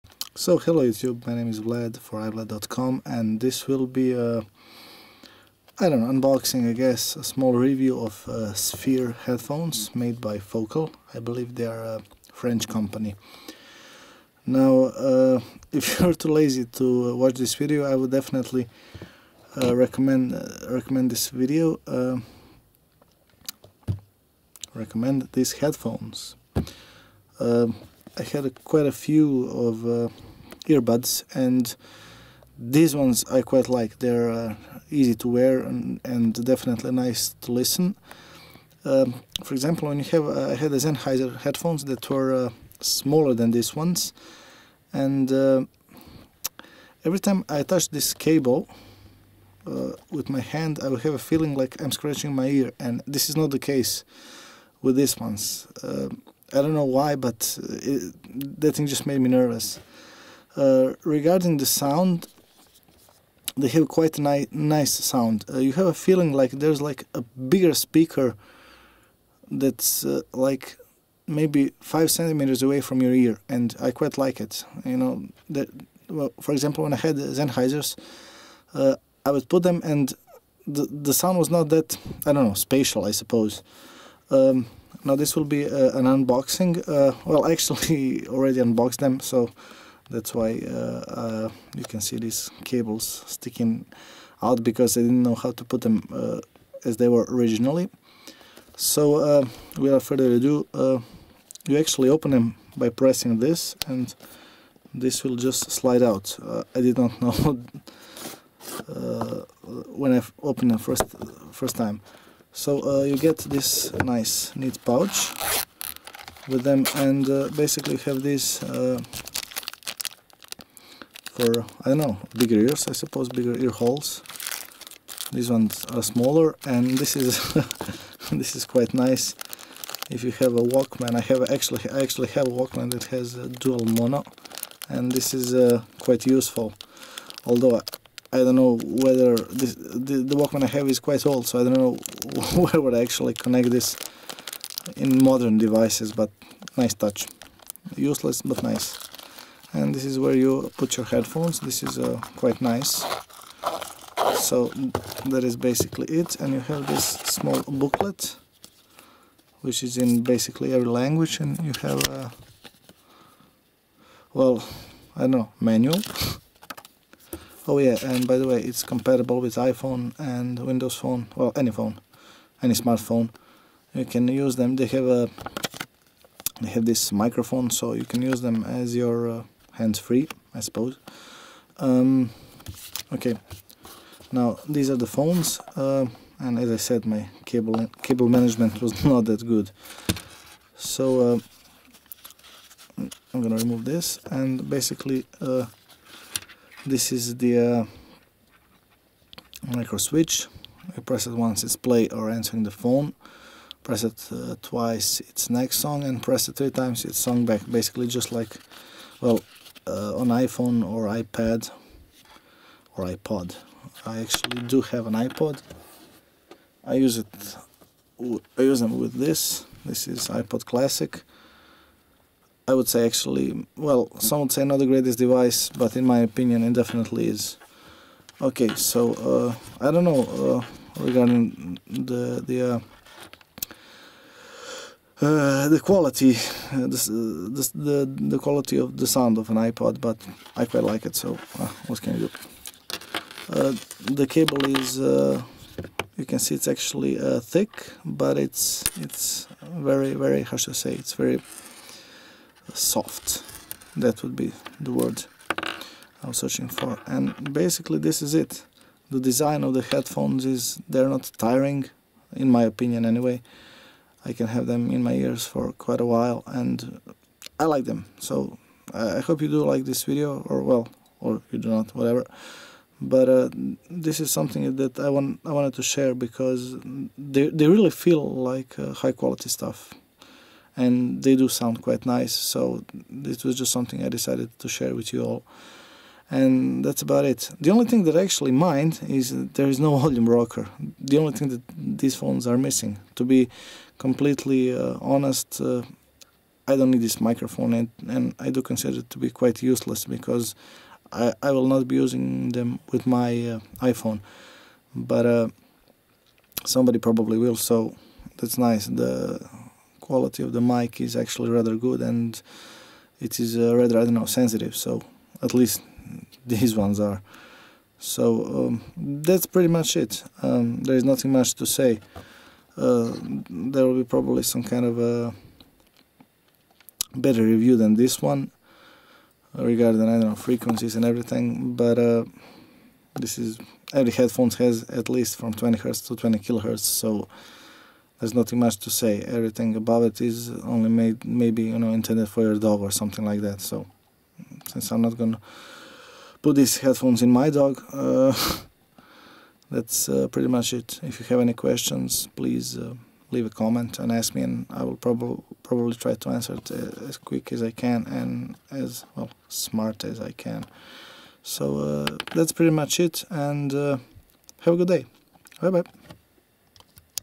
[HW: Review] - Focal Sphear (In-Ear headphones)
TLDR:

Good:
Nice sound quality
Good frequency response (20Hz-20KHz)
Comfortable
Not tiresome

Bad:
No volume rocker (it would be nice if it were incorporated in microphone)
Maybe price

UPDATE (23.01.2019) : For no reason play/pause button stopped working. well that sucks :/

if you find this useful, you are rich as F**K and want to pay me a beer:

BTC-ME:
1P7drxDQALyJDm8rCVSuhEXkiS5LnYFpRr

ETH-ME
0x21E4cC6BEf890000B1cD52e23F426382d5651B92